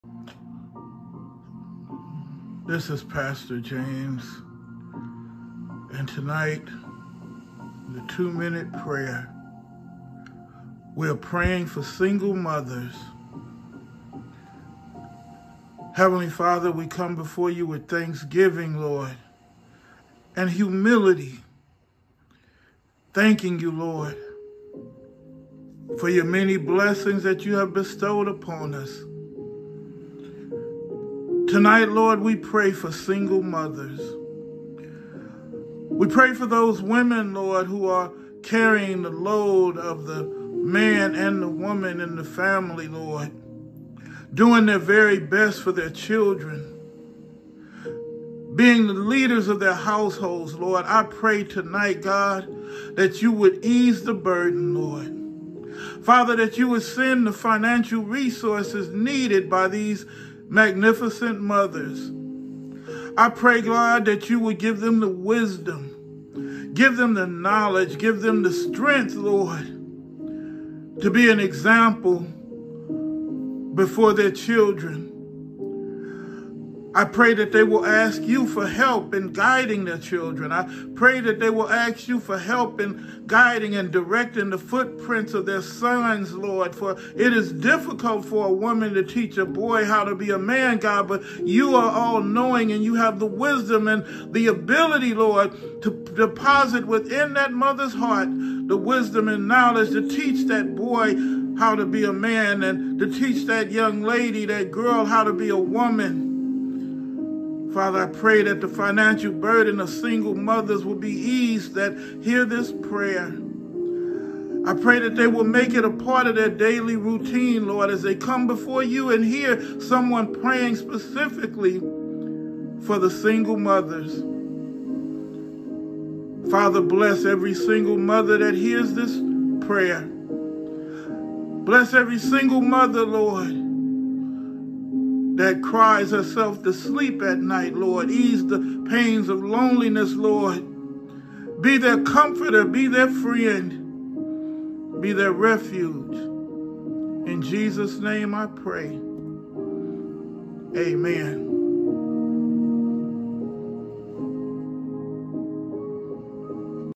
Pastor James - Let’s Pray for Single Mothers.
This edition of the “2 Minute Prayer” is dedicated to single moms. Please share with a single mom who needs encouragement.